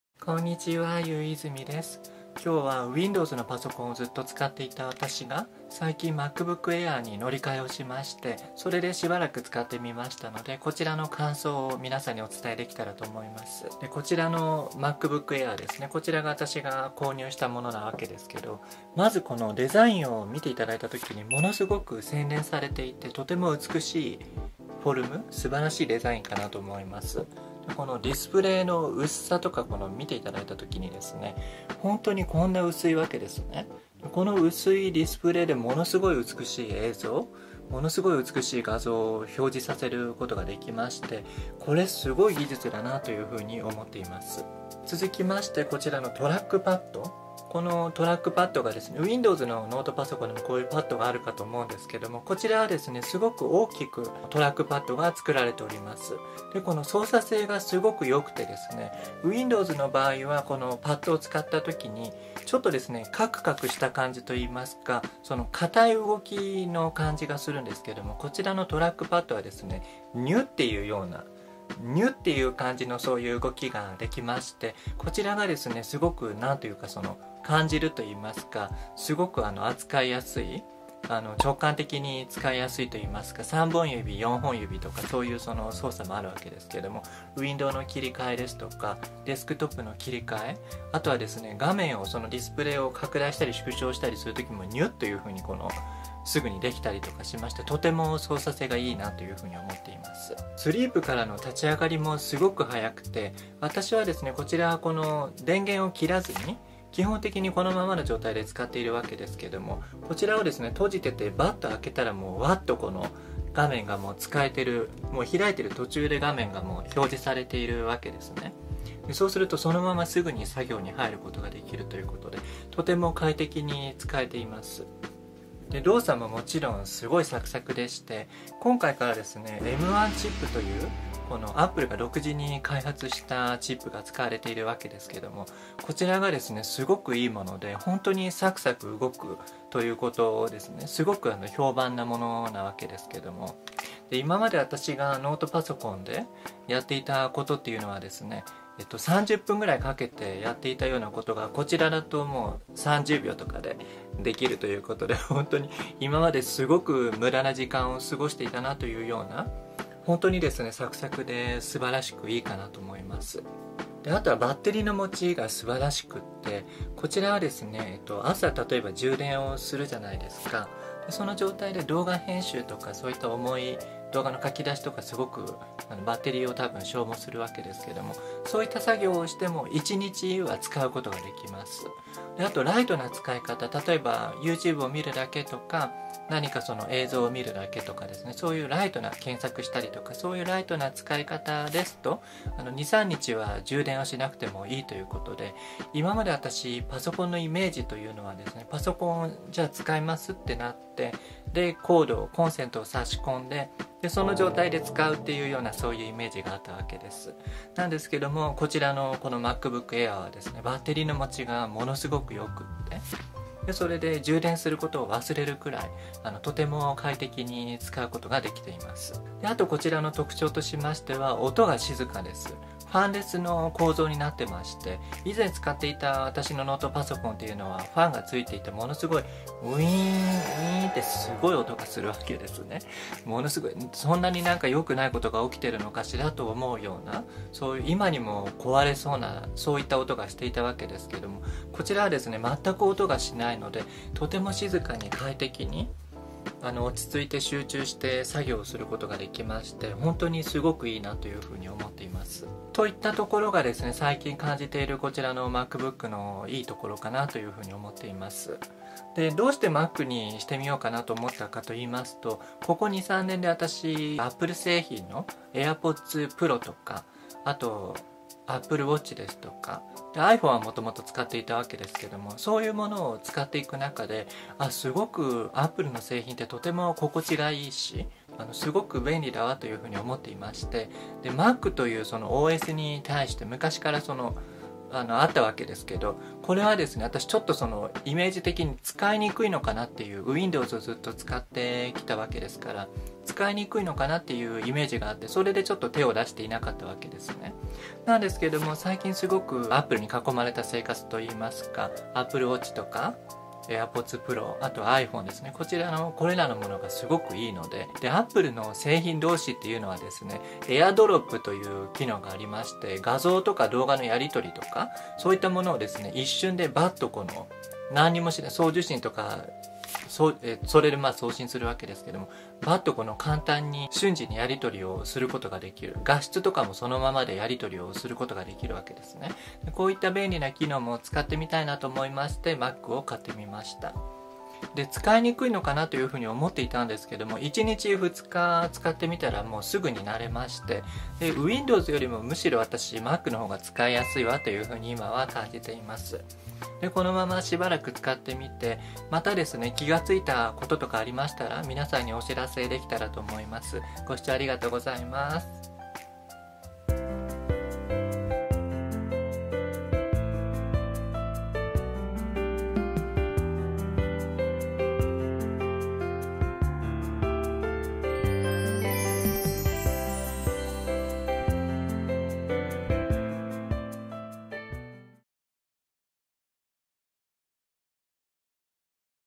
【M1 MacBook Air】Windowsユーザーの私がM1MacBookAirに乗り換えてしばらく使った感想！【神】
もっと早くMacを使っていたら良かったと思います。
とても快適に使用できていますよ☆